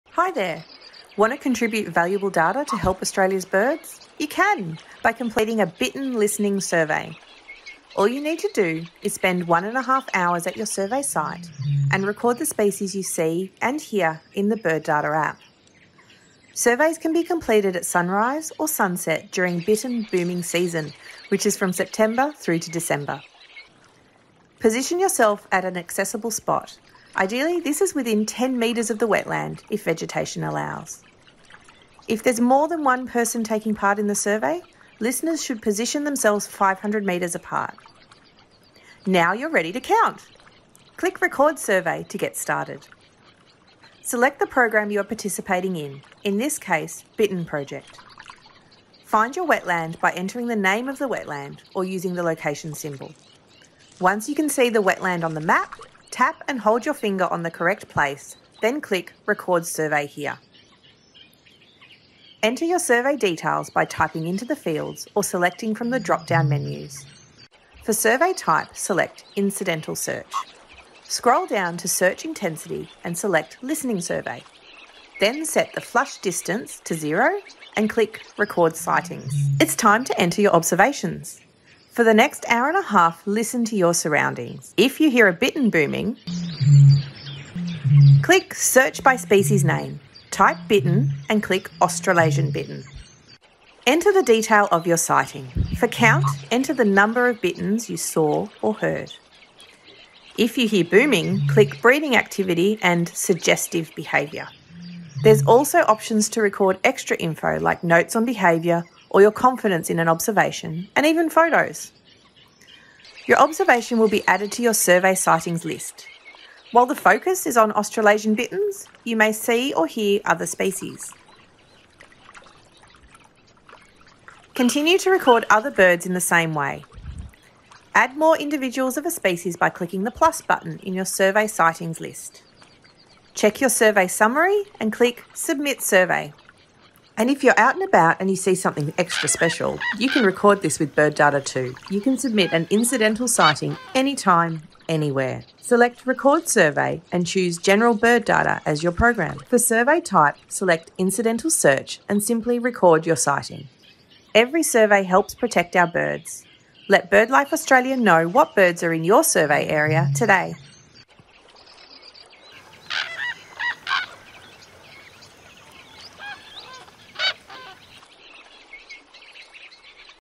How to submit a BirdLife Australia survey in Birdata | Bittern Listening Survey
Want to contribute valuable data to help Australia’s birds? Follow along with us today as we show you how to complete a BirdLife Australia Bittern Listening Survey.

All you need to do is:
• spend 1.5 hours at your survey site or wetland,
• and record the species and number of birds you see in the Birdata app.

Every survey helps to protect our birds. Let BirdLife Australia know what birds you see today!